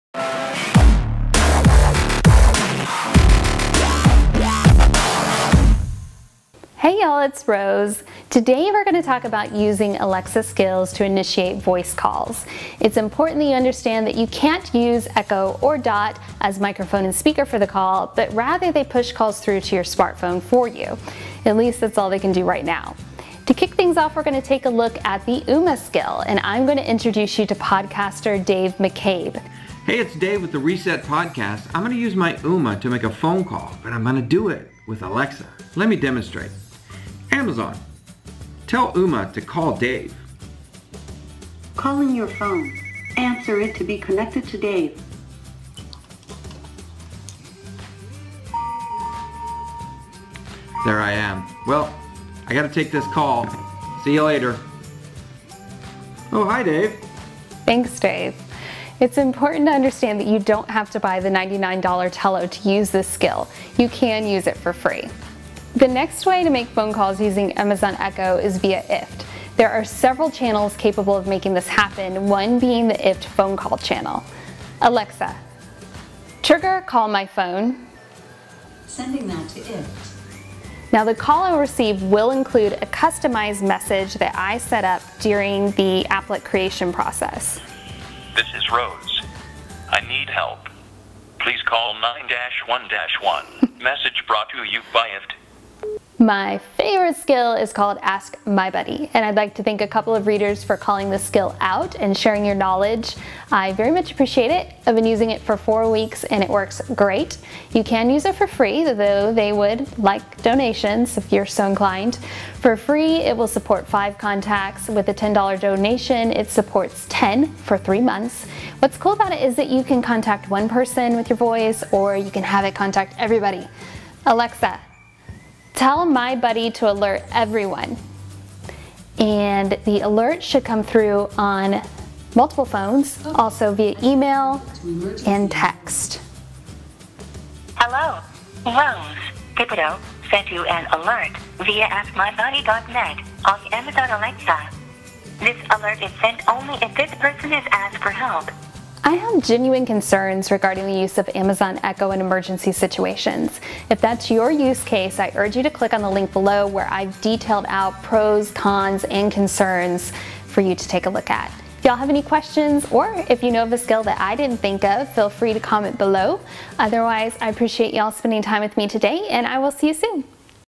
3 Ways Amazon Alexa Can Initiate Emergency Calls
The video includes a brief overview of three skills that allow voice activated calls using Alexa-enabled devices like Amazon Echo, Echo Dot, and more. Services reviewed include Ooma, IFTTT, and Ask My Buddy. Ooma and My Buddy can be activated within the Alexa app. Click on the settings tab on the left side of the app, select skills, search for either Ooma or Ask My Buddy, and enable the appropriate skill. To use IFTTT, you will need an IFTTT account and then you will need to activate the IFTTT phone call channel

As mentioned in the video, I have serious concerns regarding the use of Alexa in emergency situations.